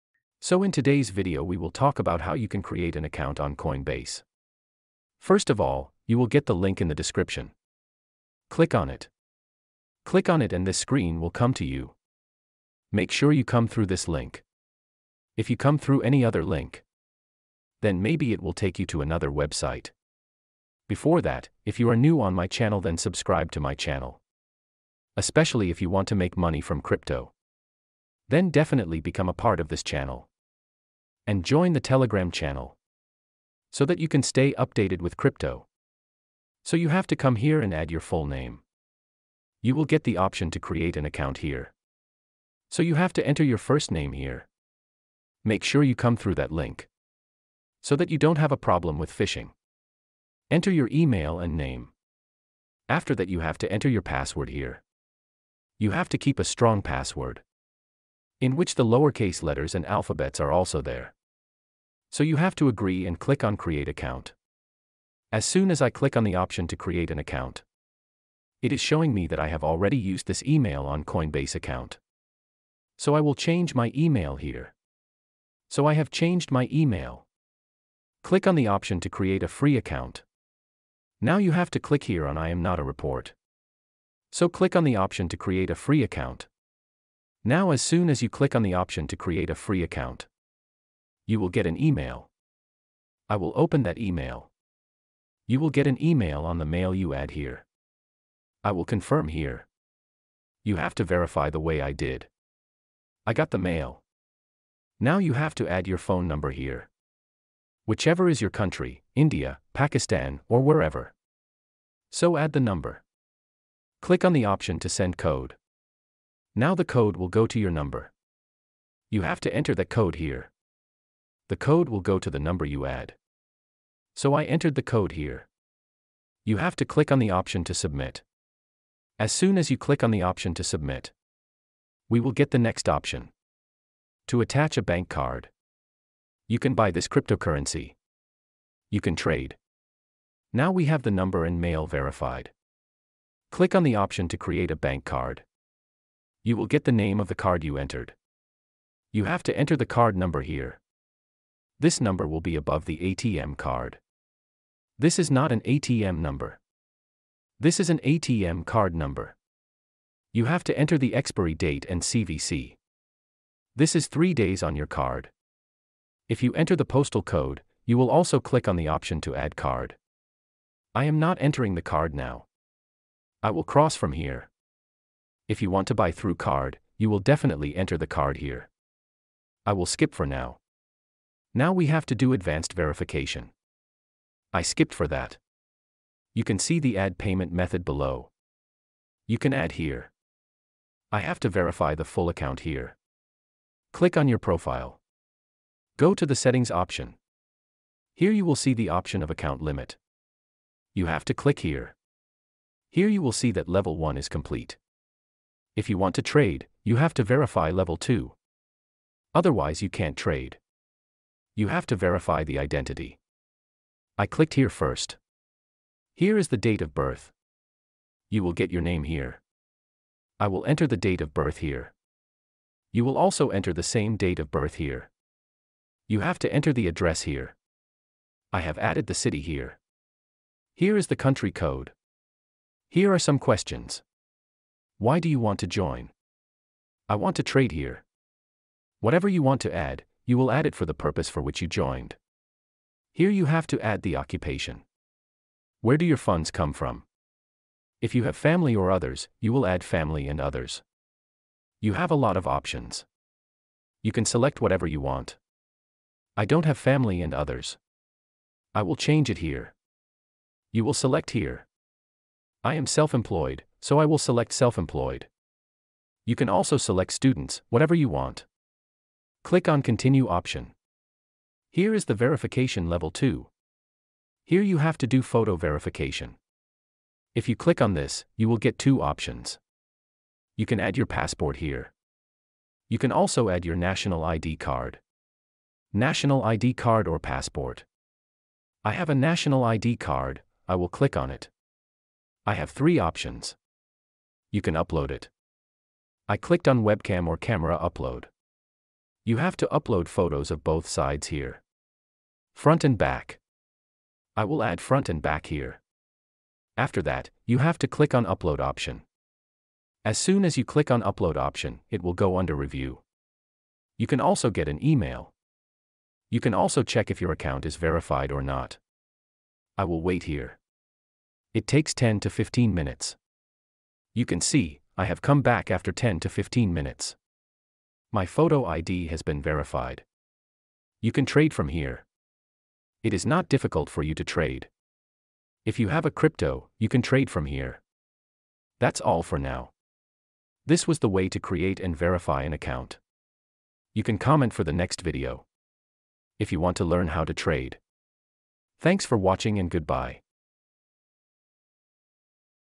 How to create coinbase account 2023 / Step by step guide
Coinbase is a secure and reliable platform for buying, selling, and storing cryptocurrencies. It is one of the most popular cryptocurrency exchanges in the world, with over 30 million users. Coinbase is regulated by the U.S. Securities and Exchange Commission (SEC) and the Financial Crimes Enforcement Network (FinCEN).

Learn how to create a Coinbase account in this video. Coinbase is a popular cryptocurrency exchange that allows you to buy, sell, and store cryptocurrencies.